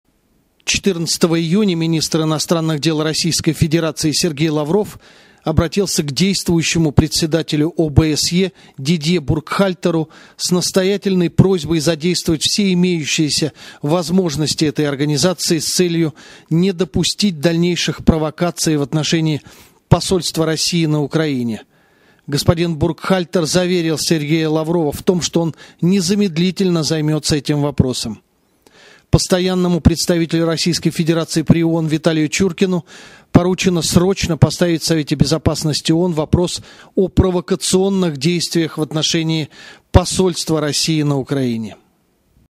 Заявление МИД России о провокации против Посольства России в Киеве 1426-14.06.2014
Заявление МИД России по ситуации вокруг Посольства России в Киеве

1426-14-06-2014

14 июня Министр иностранных дел Российской Федерации С.В.Лавров обратился к Действующему председателю ОБСЕ Д.Буркхальтеру с настоятельной просьбой задействовать все имеющиеся возможности этой Организации с целью не допустить дальнейших провокаций в отношении Посольства России на Украине.

Д.Буркхальтер заверил С.В.Лаврова в том, что он незамедлительно займется этим вопросом.

Постоянному представителю Российской Федерации при ООН В.И.Чуркину поручено срочно поставить в Совете Безопасности ООН вопрос о провокационных действиях в отношении Посольства России на Украине.